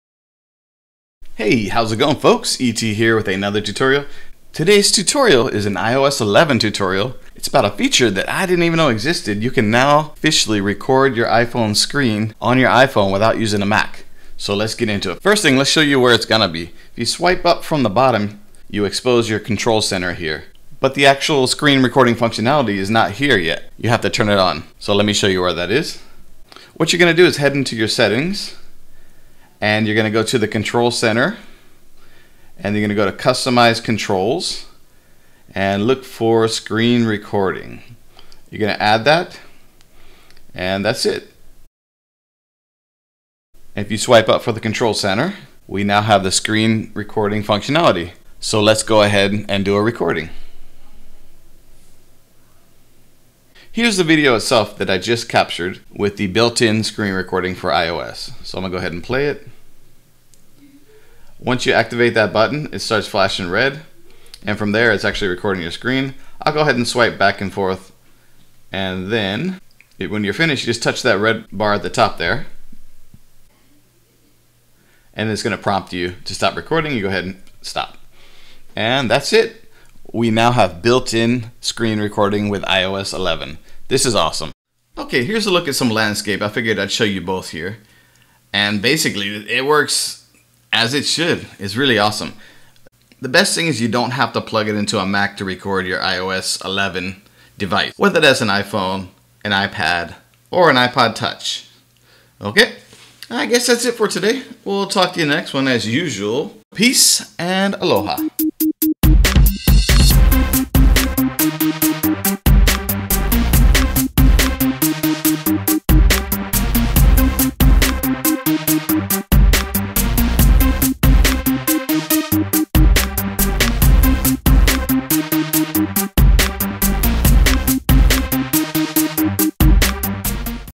How To Record Your iPhone Screen with iOS 11 - Tutorial
In this iPhone Tutorial I show you how to record your iPhone screen with the built in screen recorder that comes with iOS 11. It doesn't display by default so you have to turn it on to get it to appear in your control center.

ET